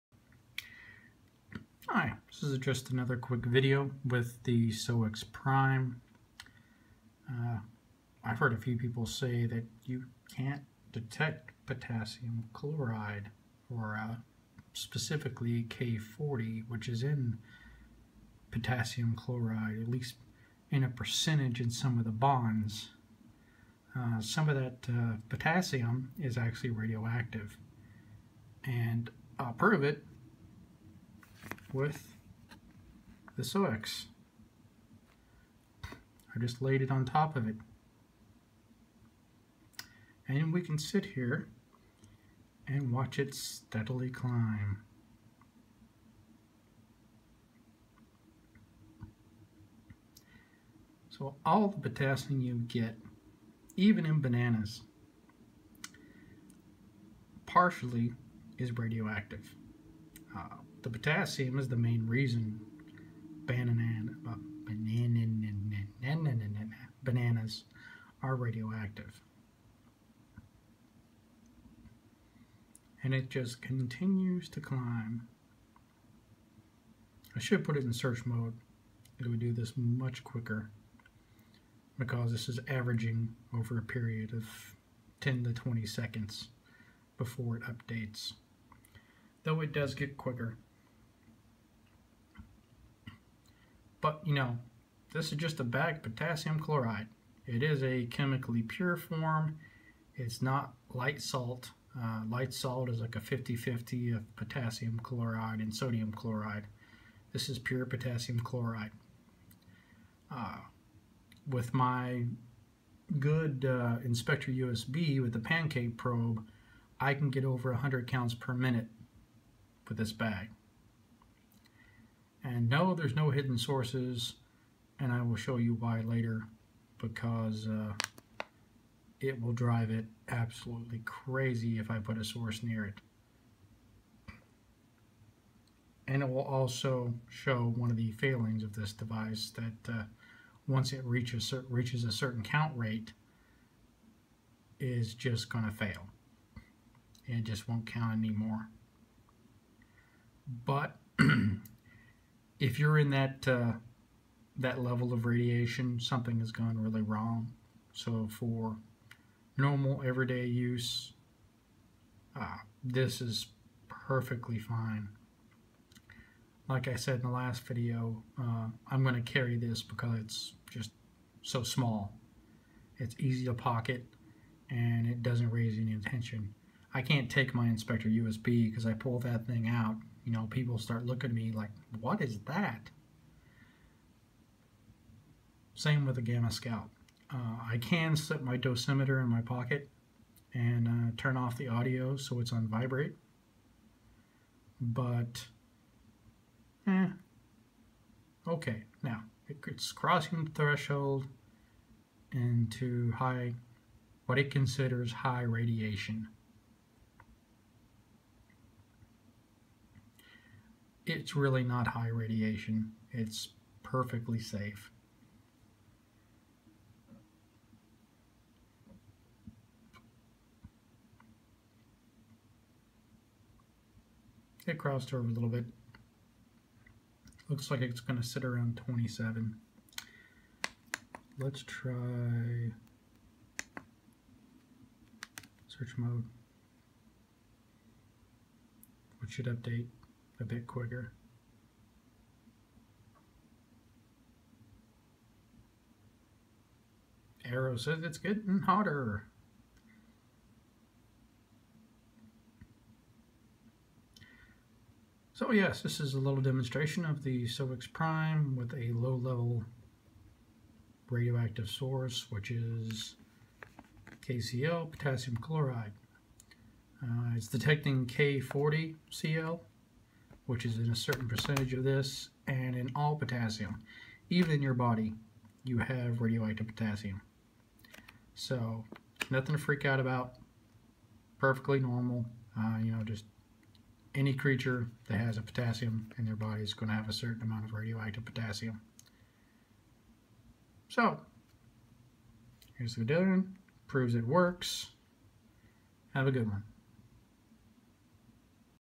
Soeks Prime detecting radioactive potassium
Using the Soeks Prime to show Potassium Chloride is a low level radioactive substance, and that the Soeks Prime can detect it despite the smaller geiger muller tube.